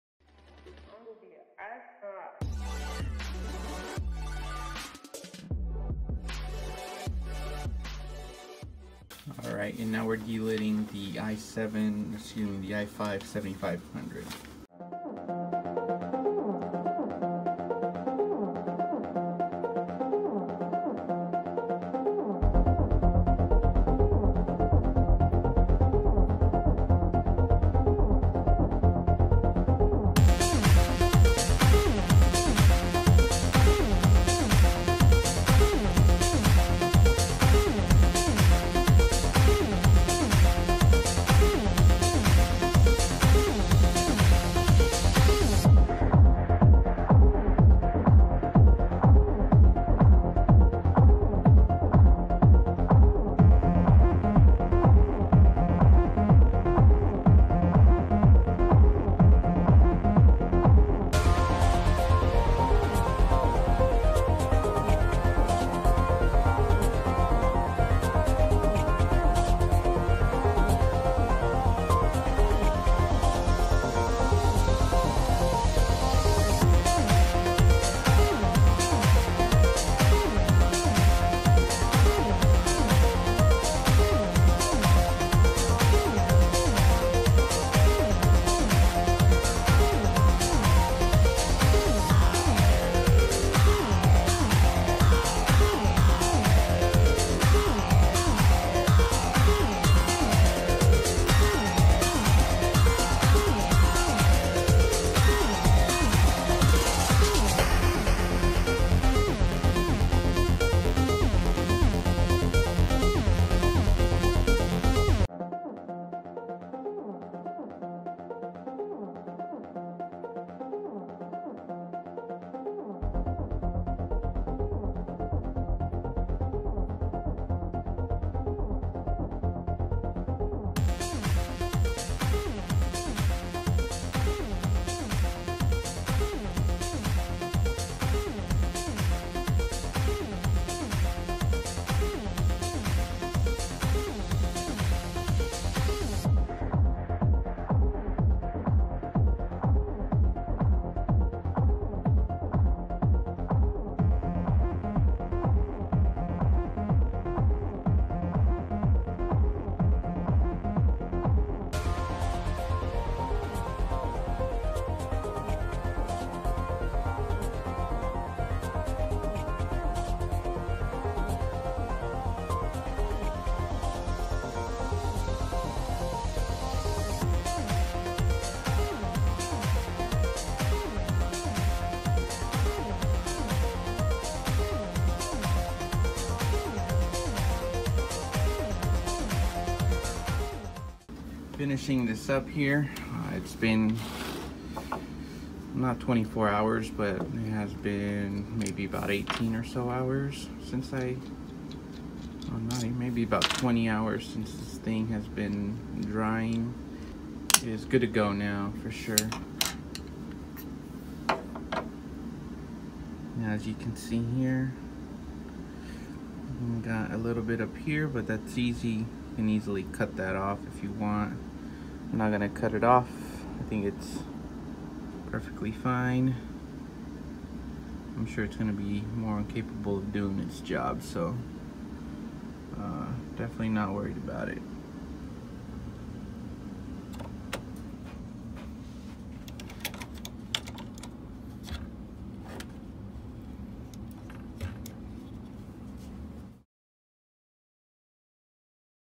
Delidding i5 7500 & Liquid Metal
This is a quick video on delidding an Intel i5-7500 and applying liquid metal. Short note - Liquid Metal not needed for this CPU